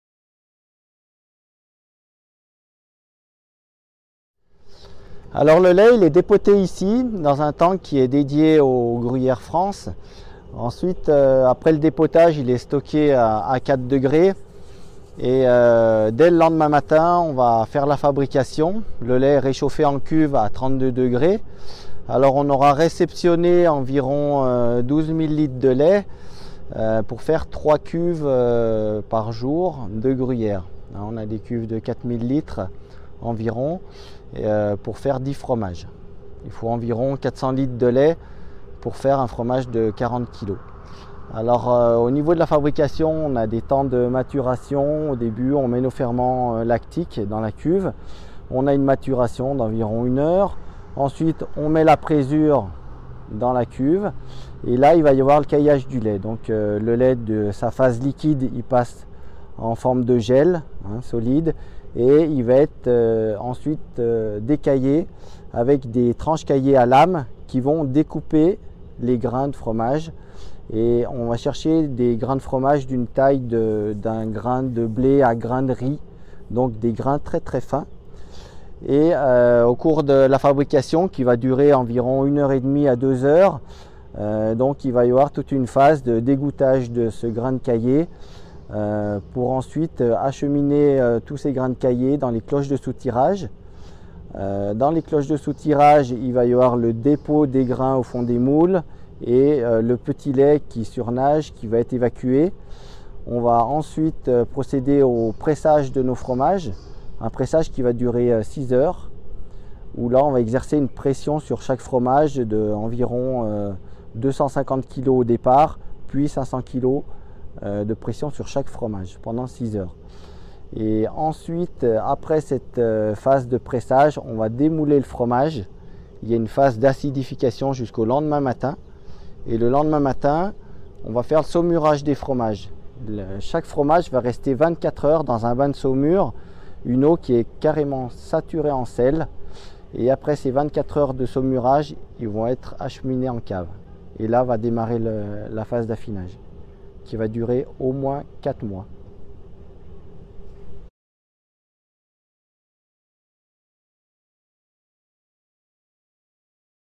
12 000 litres de lait arrive quotidiennement à la fromagerie
Quand le lait arrive à la fromagerie...
Suivez les étapes de fabrication du Gruyère de France.
Chaque jour, ce sont 12 000 litres de lait qui arrive à la fromagerie de Saint-Germain La Chambotte pour fabriquer 10 meules de Gruyère IGP de 40kg.